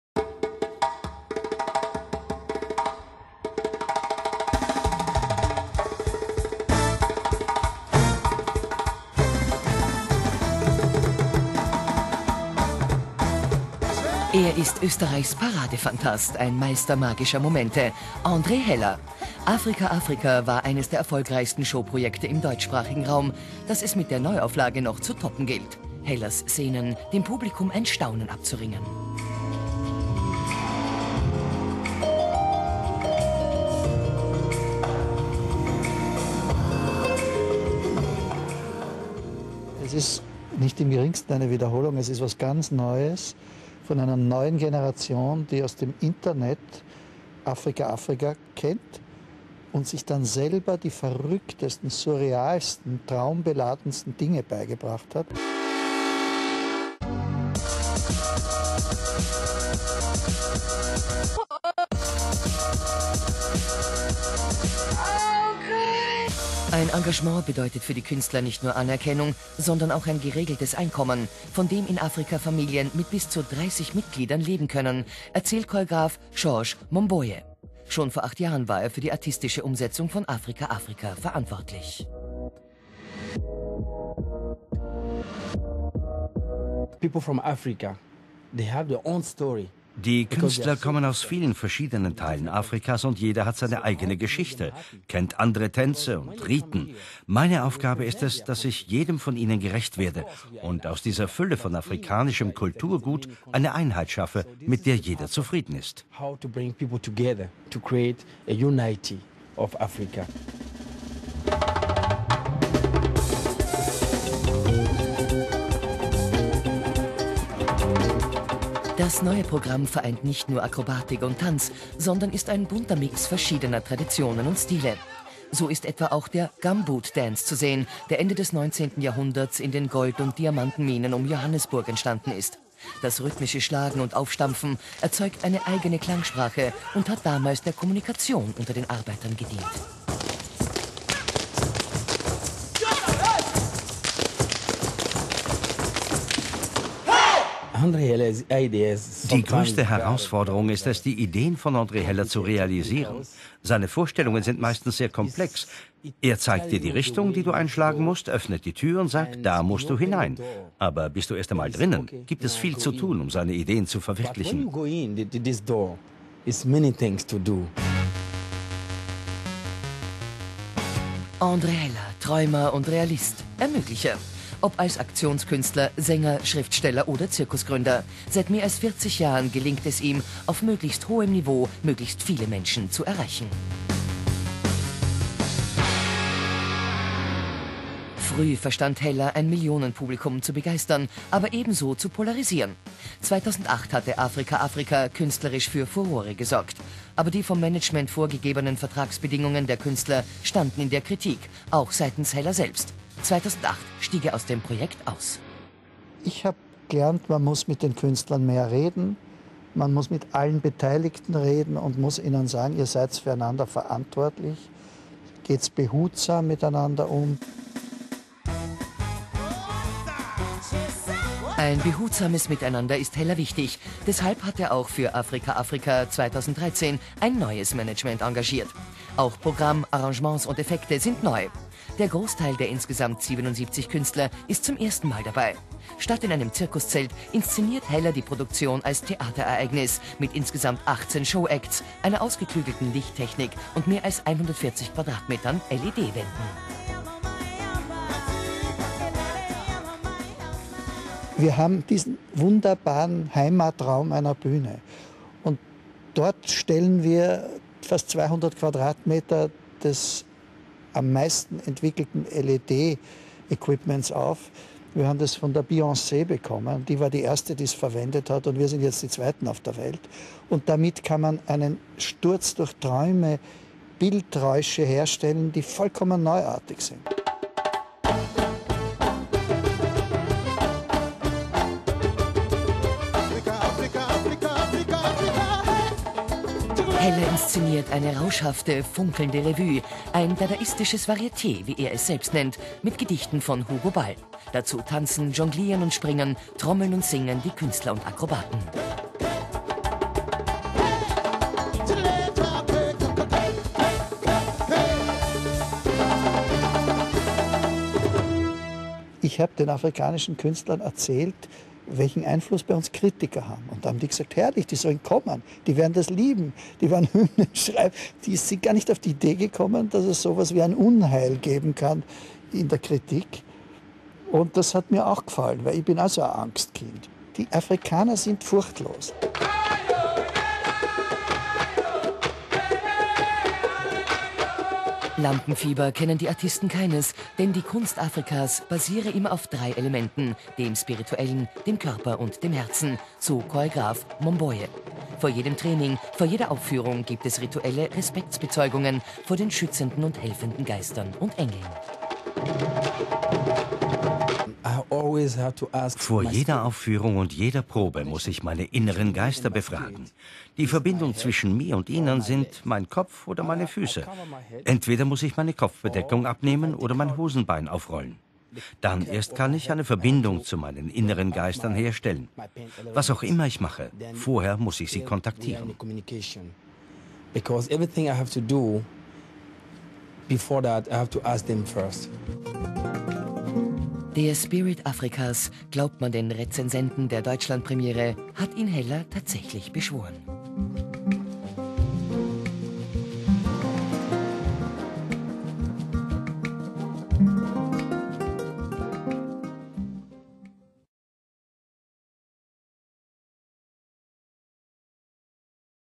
André Hellers Afrika! Afrika! 2013 - DAS PORTRÄT
Die Show

André Hellers Afrika! Afrika! feierte Ende September eine glanzvolle Premiere im Festspielhaus Baden-Baden. Die Kritiken sind euphorisch, Feuilleton und Publikum gleichermaßen begeistert von der energetischen und zugleich eleganten Ausstrahlung und Kraft dieses multimedialen, farbenfrohen und fröhlichen Gesamtkunstwerks. André Heller Zirkusproduktion Afrika! Afrika! gilt als einer der größten Erfolge im internationalen Showbusiness der vergangenen 20 Jahre. Mit seiner Neuinszenierung von Afrika! Afrika! ist André Heller, zusammen mit seinem Team von Choreographen, Lichtkünstlern und Artisten, eine opulente Neufassung von Afrika! Afrika! für die Bühne gelungen. Spielfreude, Artistik, Jonglage, Tanz und atemberaubende Kunststücke. Tempo, Farbenrausch und pure Lebensfreude -- Afrika! Afrika! erobert die Bühnen und die Herzen des Publikums. Die aktuelle Tour hat auch prominente Unterstützung: Der aus dem Senegal stammende Musiker Youssou N'Dour hat offiziell seine Patronanz für das Showprojekt bestätigt. Besser kann Völkerverständigung nicht zelebriert und gelebt werden